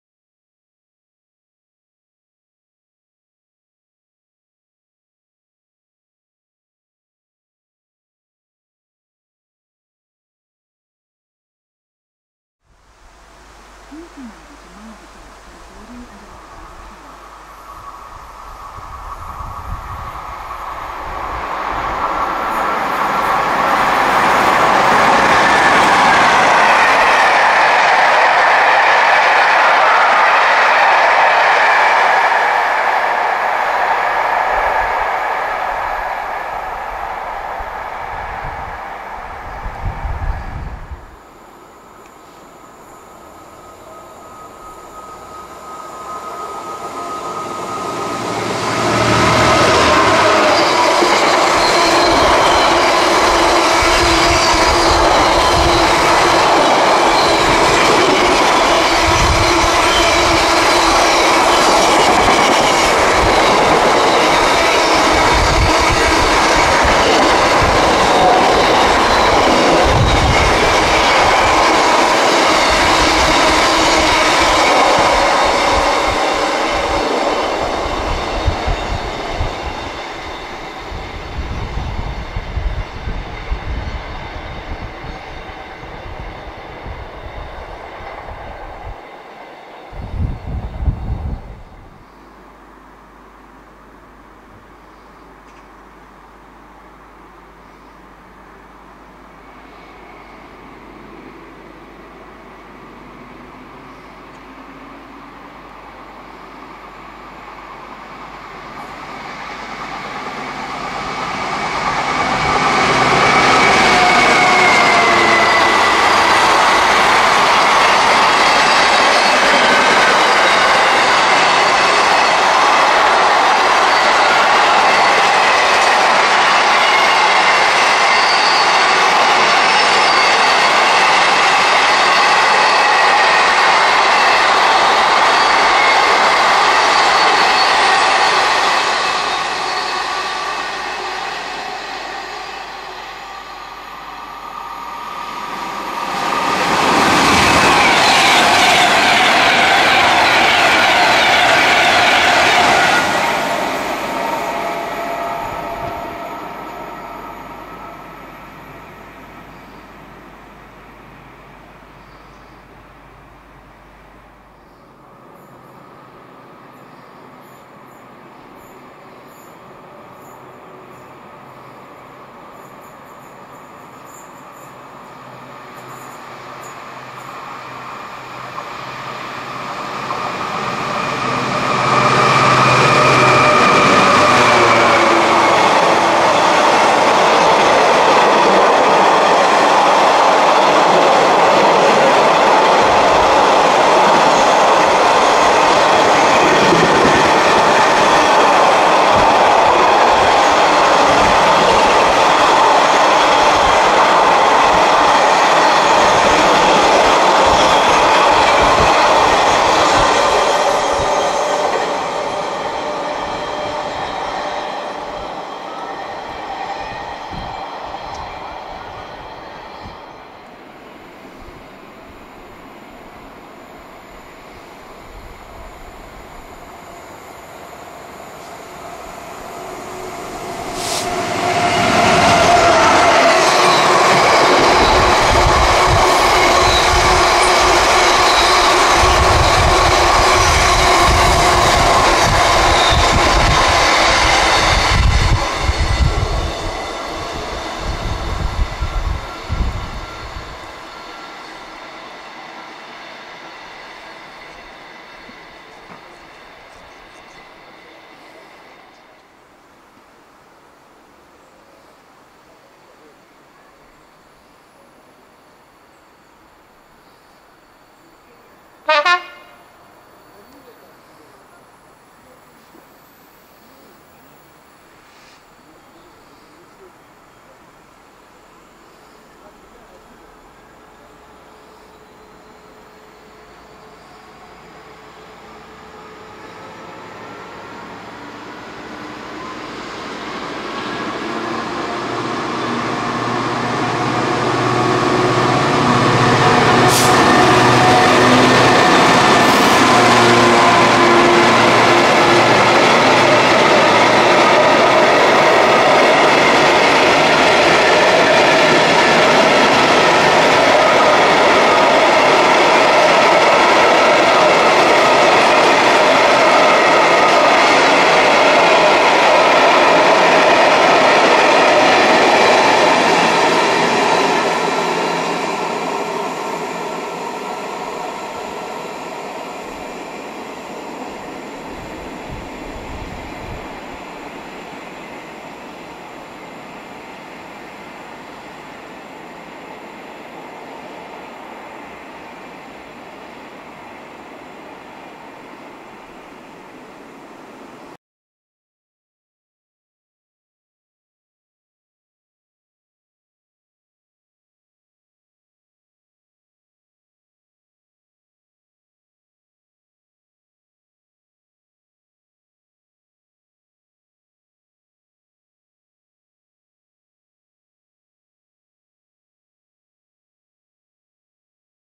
Lichfield Trent Valley & Tamworth - Wednesday 10th July 2024 - Part 1/3
'Interesting Stuff'

Continuing my quest to revisit places I hadn't been to for many years...a ride up to Lichfield Trent Valley on a 730 unit from New St, then down to Tamworth for a few hours running up and down the stairs to try and film as much as possible.

Tamworth, as ever, was excellent - I've posted 47848 already, but included it here again for completeness.